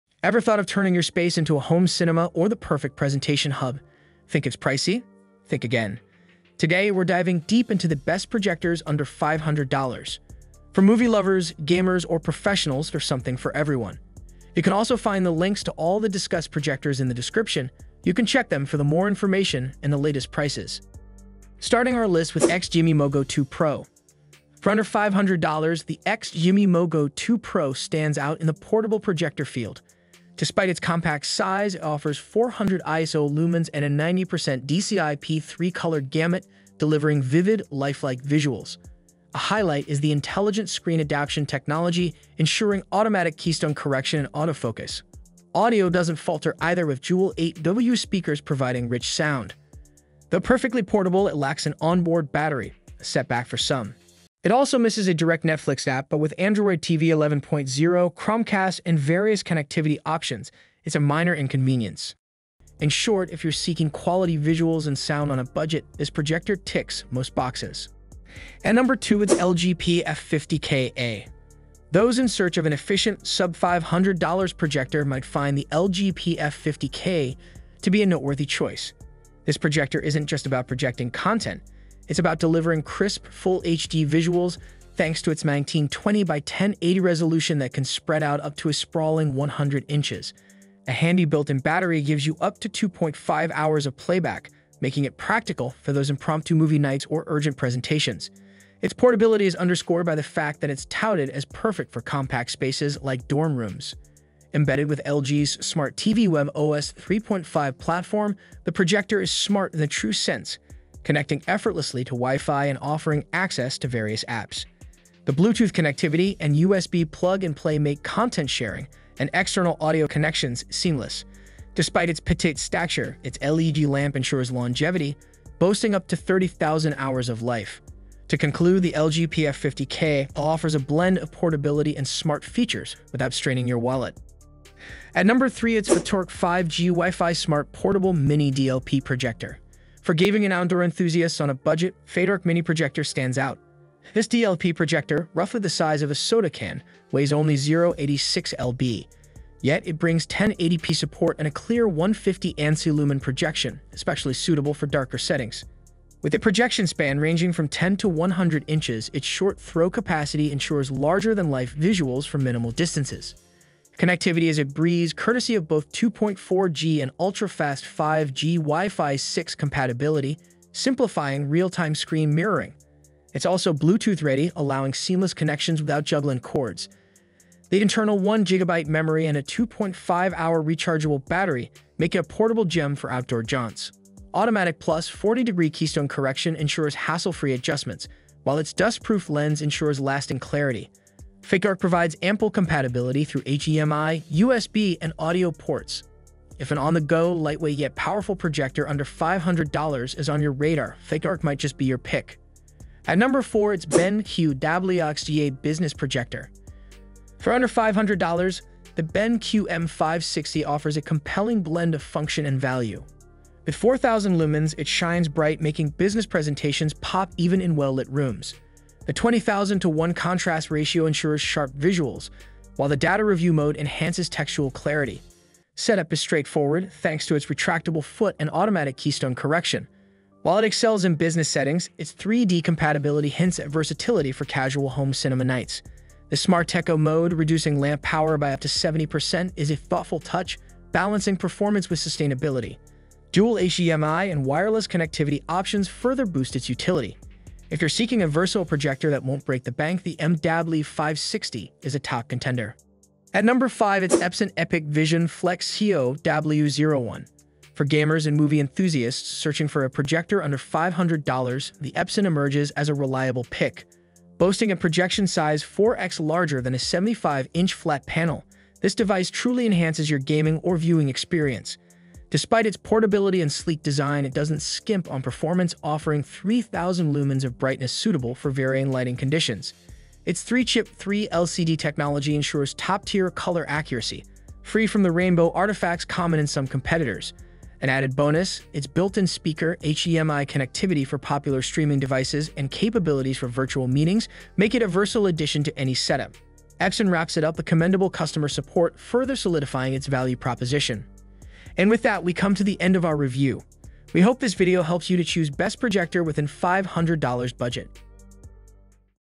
The 5 Best Projectors Under $500 in 2023/2024
Links to the Best Projector Under $500 in 2023/2024

1. XGIMI MoGo 2 Pro
2. LG PF50KA
3. FATORK 5G
4. BenQ WXGA
5.  Epson EpiqVision Flex CO-W01

🎥 Are you in search of the best projector that won't break the bank? Look no further! In today's video, we're rounding up the Top 5 Best Projectors that you can grab for under $500. Whether you're setting up your home cinema, gaming den, or need something for your presentations, we've got you covered.